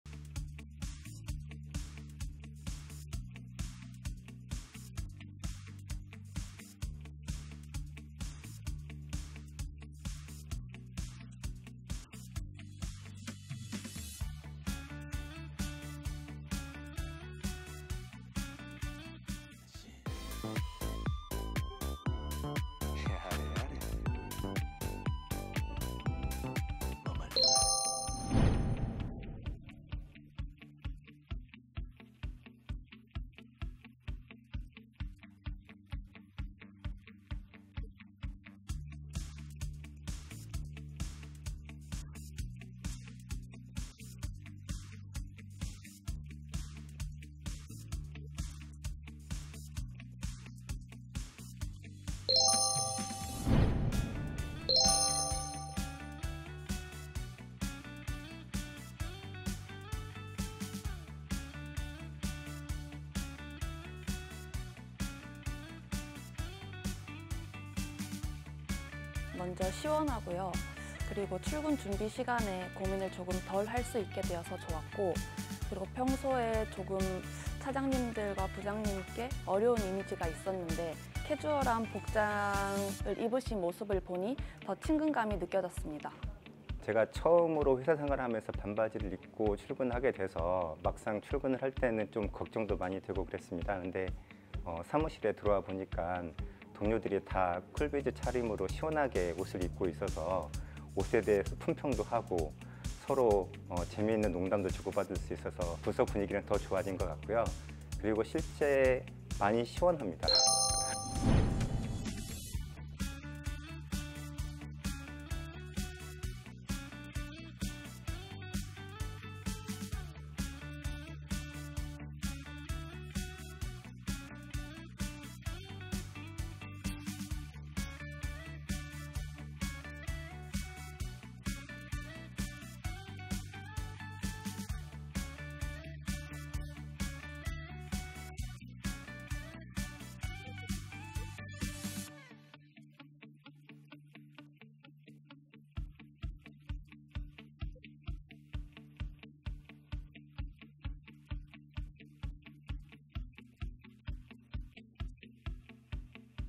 우리는 반바지 입고 출근한다! 동서발전 여름맞이 쿨비즈 근무 시작
한 여름에 갑갑한 넥타이, 불편한 정장 차림
생각만 해도 넌덜머리가 난다구요?!
동서발전에선 반바지에 편한 티셔츠 입고도 일만 잘한다는 사실!

더 시원한 차림으로 생각도 문화도 시원해지는
동서발전의 여름맞이 쿨비즈 근무!

한여름 더위도 무섭지 않은, 시원한 근무처 동서발전 모습
지금 바로 공개합니다!